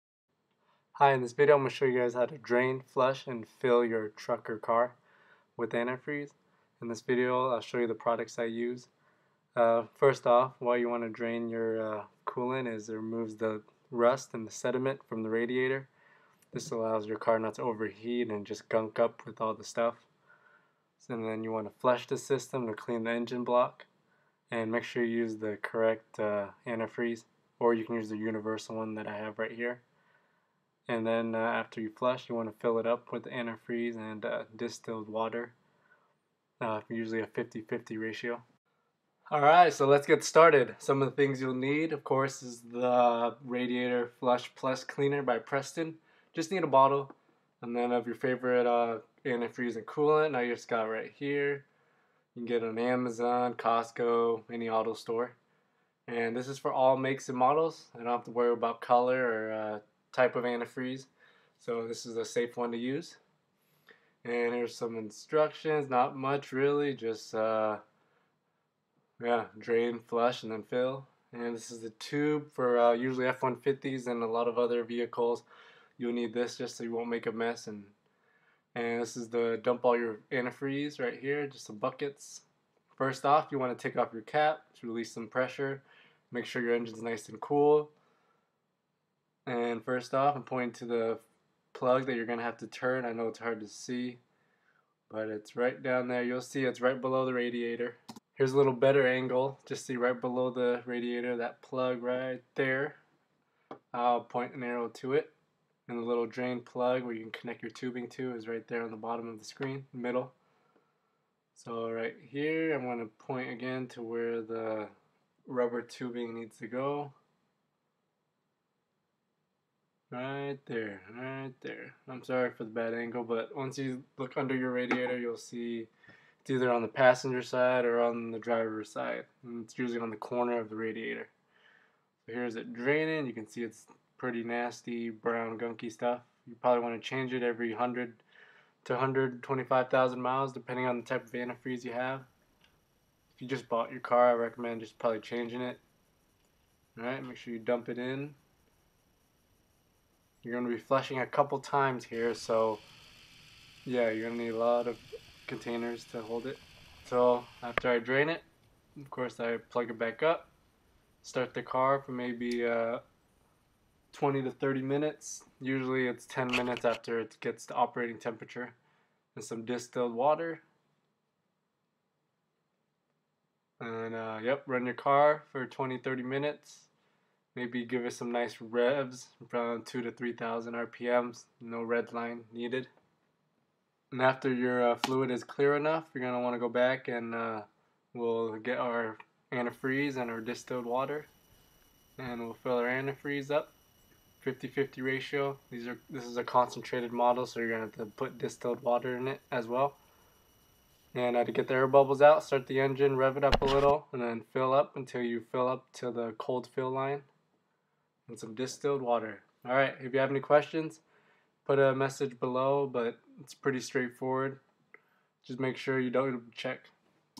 Tutorial: Properly Flush Radiator Coolant
Click on AMAZON links below for CHEAPEST prices on the items required for this project.

Prestone Radiator Flush - under $3

Chevron Antifreeze (made for all vehicles) - $9

Oil Funnel - $4

Distilled water - your local grocery store - $1 per gallon

Draining Tube - local auto store - $1 / foot

Amount: see owner manual, will vary.

Total cost around $50. SAVE you lots of money to do it at home and yourself. Only needs to be done once every 3-5 years.